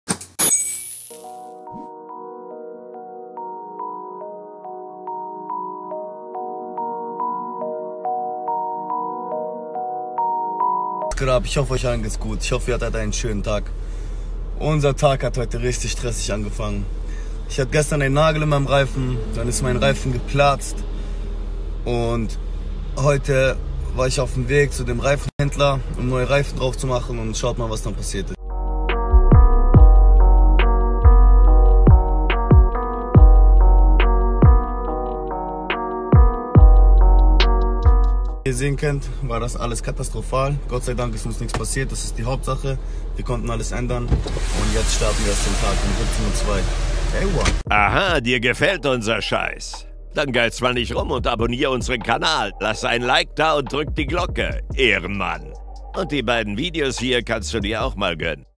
Reifen von Nimos Auto geplatzt | DeinUPDATE
Beim Fahren ist einer der Reifen von Nimos Auto geplatzt. Gott sei Dank wurde keiner verletzt!